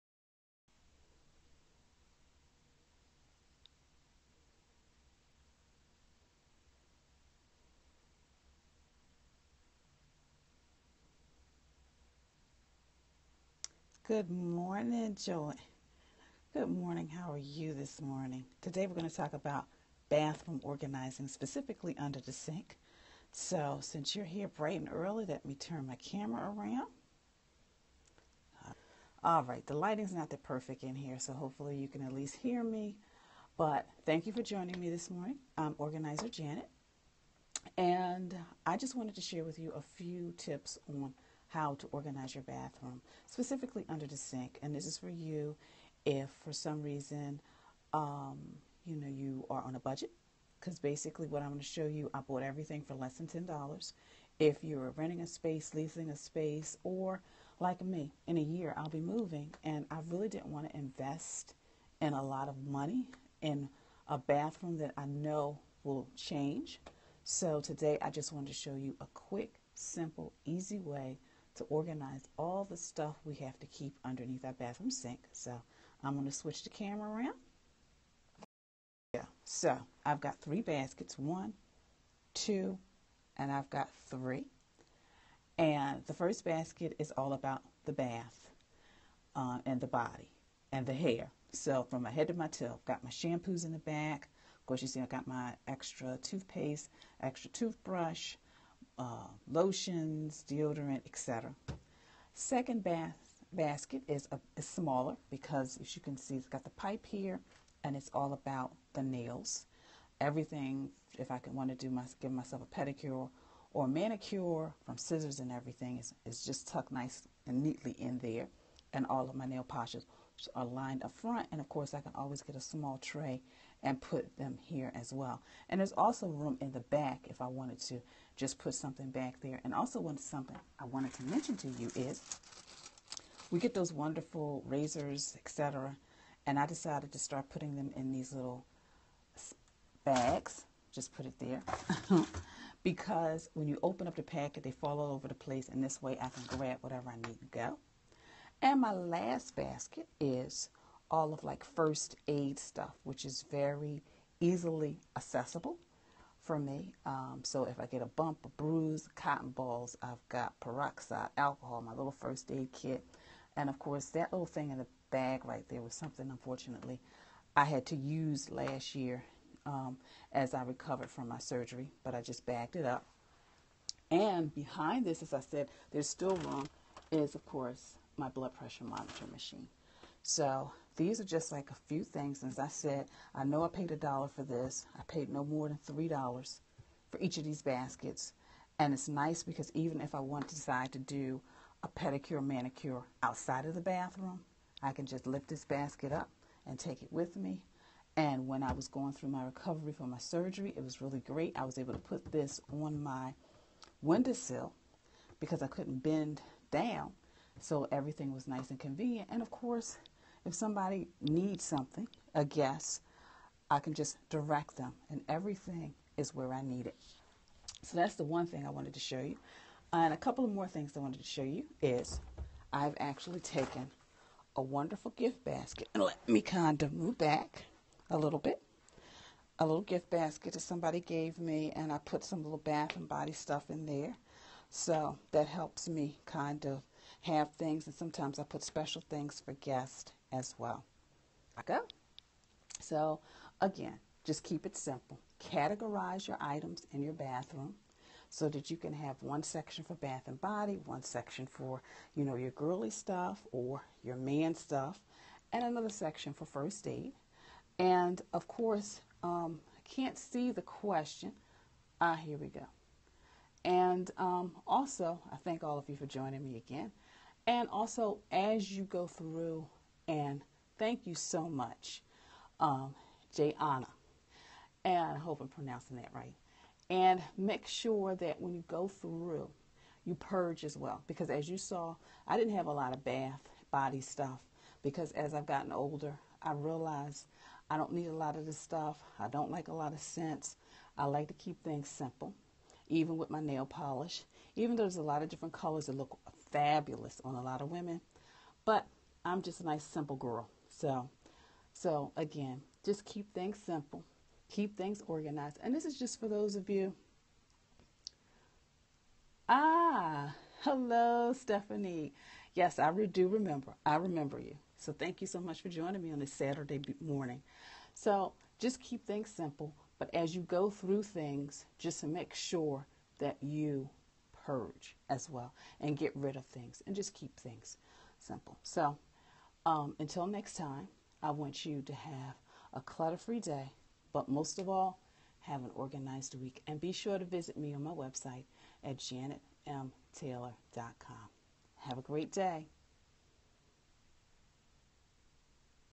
trimOrganizerJanet Under the bathroom sink organizing tip
I share a quick tip on how to organize underneath the bathroom sink.